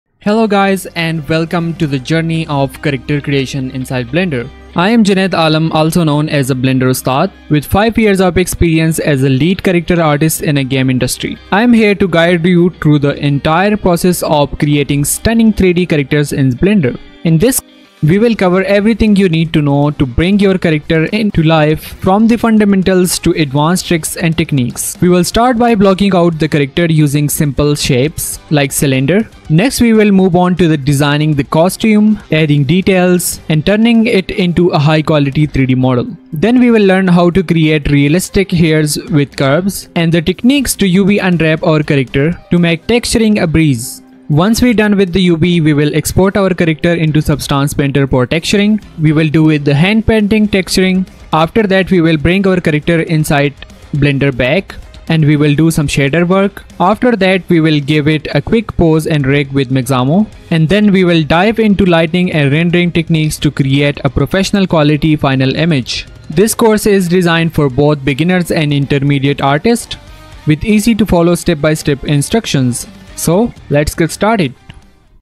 3D Character Creation : From Modeling to Rendering | Course Trailer
Learn to create detailed 3D characters from scratch, Mastering Modeling, Texturing, Rigging, and Rendering using Blender

In this comprehensive modeling course for beginner, you'll learn how to create a fully detailed 3D character from scratch, using Blender, Substance Painter, and Mixamo. Whether you're a beginner or looking to improve your skills, this course guides you through the entire process, from modeling to rendering, in an easy-to-follow structure.
We begin with a focus on tools and essential skills. You’ll learn about Blender for modeling, Substance Painter for texturing, and Mixamo for rigging, ensuring you have a solid foundation to start building your character. Then, we’ll dive into creating a base blockout for your character, establishing form and proportions that will serve as the foundation for adding more details.
Next, you’ll learn how to design costumes and props that complement your character, giving them depth and personality. As you move into high-resolution detailing, we’ll focus on refining anatomy and adding intricate features, resulting in a polished, professional-quality model. Hair creation follows, where you'll build the base shapes and then finalize the details, ensuring your character’s hair looks dynamic and realistic. You’ll also master UV mapping, allowing you to efficiently prepare your model for texturing without distortions.
Once the UV map is complete, you’ll export your character to Substance Painter for texturing. Here, you’ll apply colors, materials, and intricate details to bring your character to life. After refining textures with hand painting, you’ll import them back into Blender to set up shaders and run test renders, ensuring the materials look perfect in the final scene. To complete the course, you’ll rig your character in Mixamo, apply dynamic poses, and finalize it with a polished render in Blender. By the end, you’ll have a fully rigged, high-quality character ready for animation, games, or your portfolio.

This course will equip you with the knowledge and practical skills to create professional 3D characters, empowering you to take your modeling to the next level.

Blenderustad JunaidAlam blendertutorial blenderMastering Blender Sculpting | Everything You want to know about sculpting in Blender